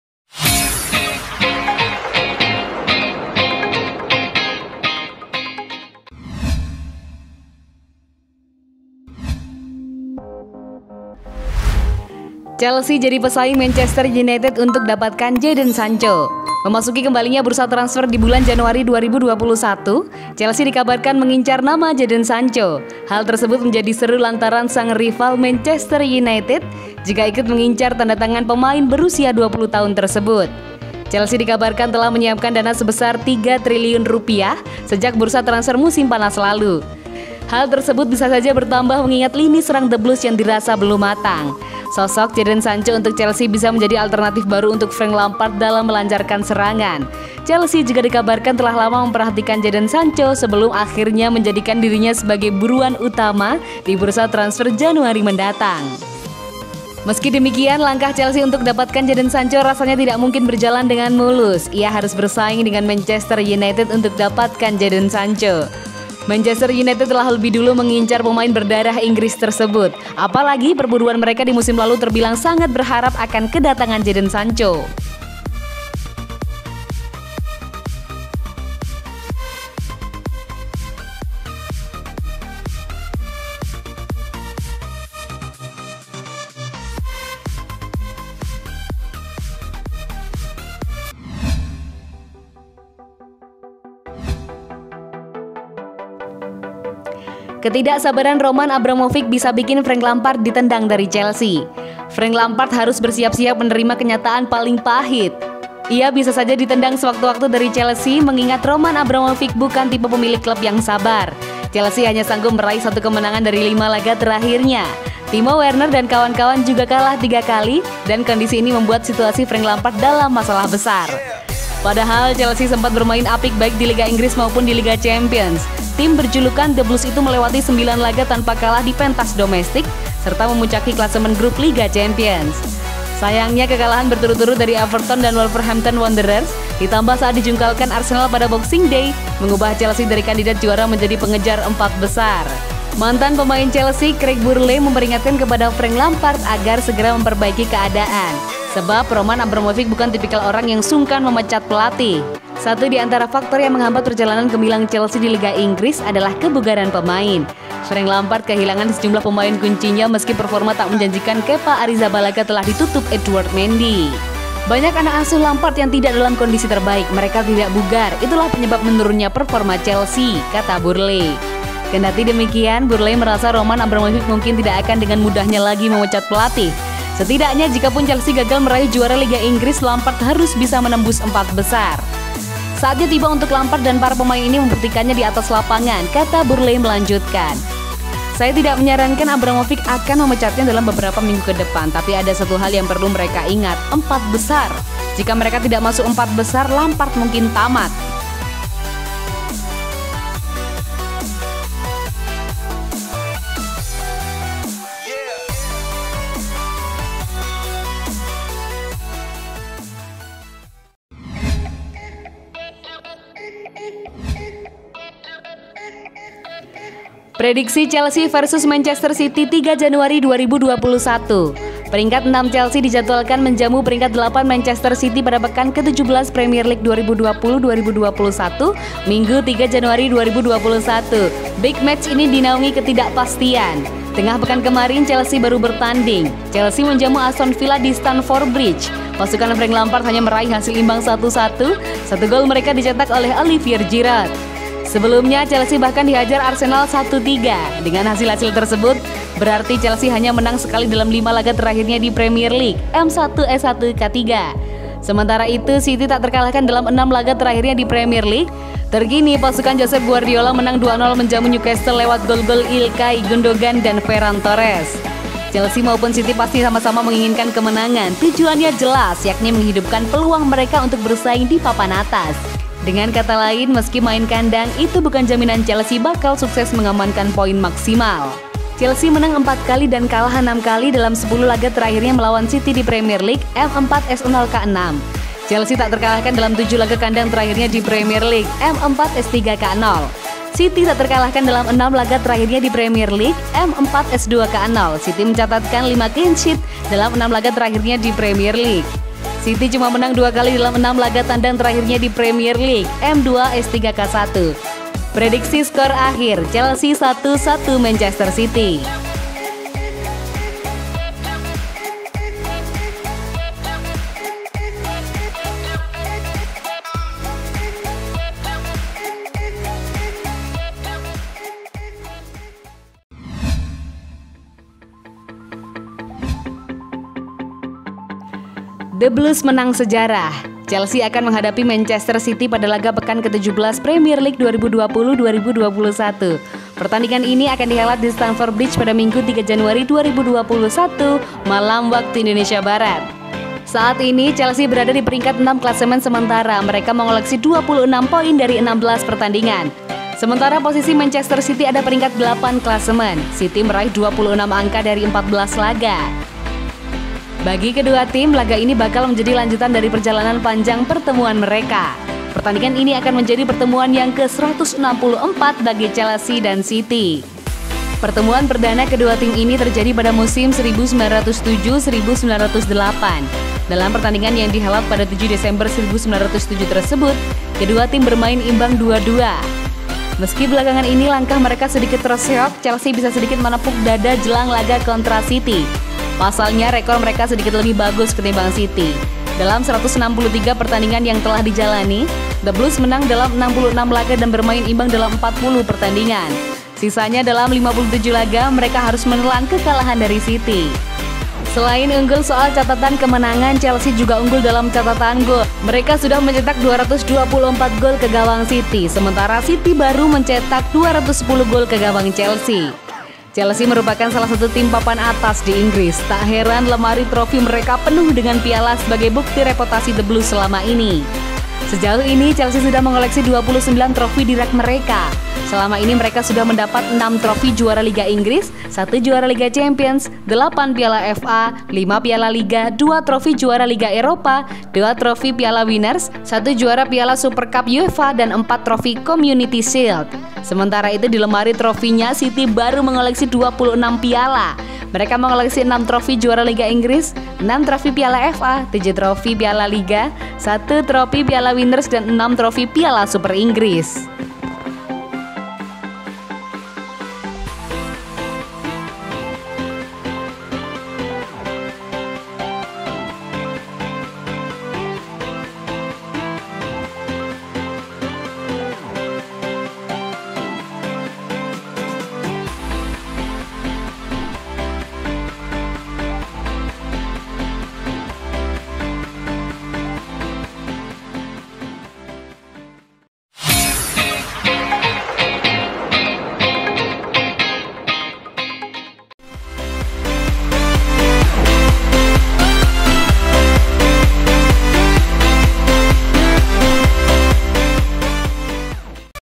Incar Jadon Sancho 🔥 Lampard Bisa Ditendang 😭 Prediksi vs Manchester City 📝💯 Chelsea Menang Sejarah
Selamat datang di channel 🔥Chelsea Fans Club🔥

- Chelsea Jadi Pesaing Manchester United untuk Dapatkan Jadon Sancho
• Ketidaksabaran Roman Abramovich Bisa Bikin Frank Lampard Ditendang dari Chelsea
• Prediksi Chelsea vs Manchester City 3 Januari 2021
• The Blues Menang Sejarah